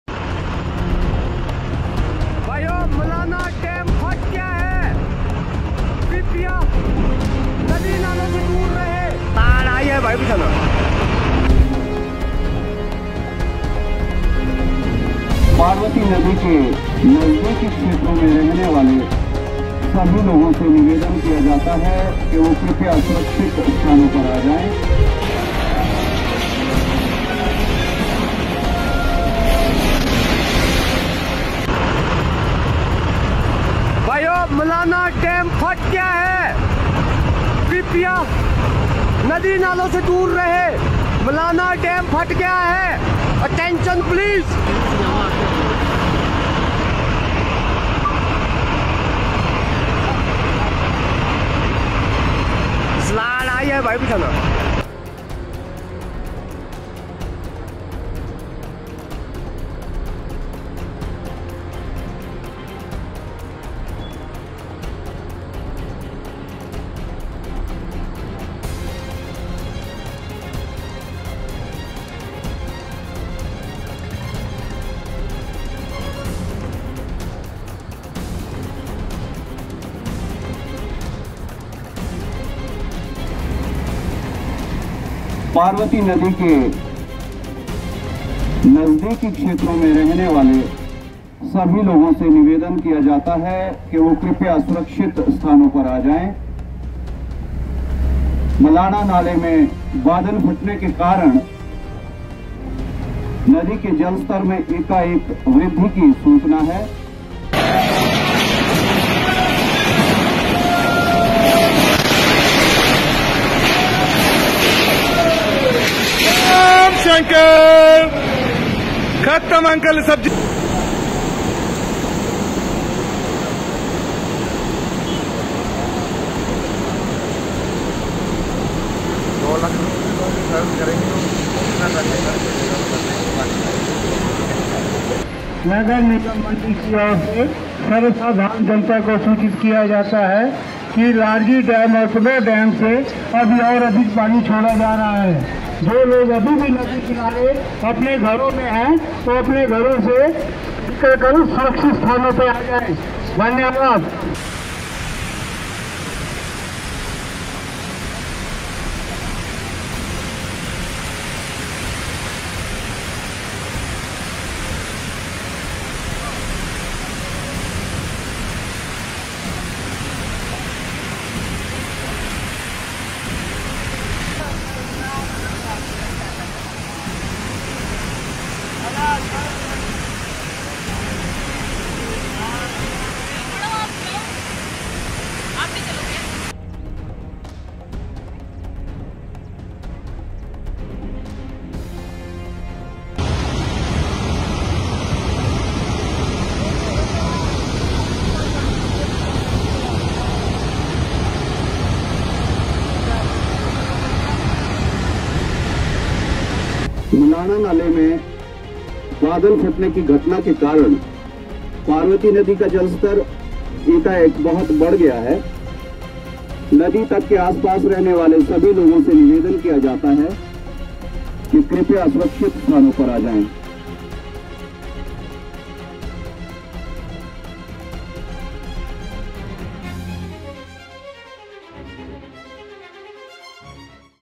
Repeat of last year in Himachal: Multiple cloudbursts, Malana dam collapses; buildings swept away
Over 20 people are missing following a cloudburst at Samej Khud in Himachal Pradesh Rampur’s Jhakri. The officials have rushed to the spot to initiate a rescue operation.
The cloudburst hit the area last night.
The regional Met office has issued an 'orange' alert for heavy to very heavy rain, accompanied by thunderstorms and lightning, in isolated places of all Himachal Pradesh districts, barring Kinnaur and Lahaul & Spiti, for Thursday.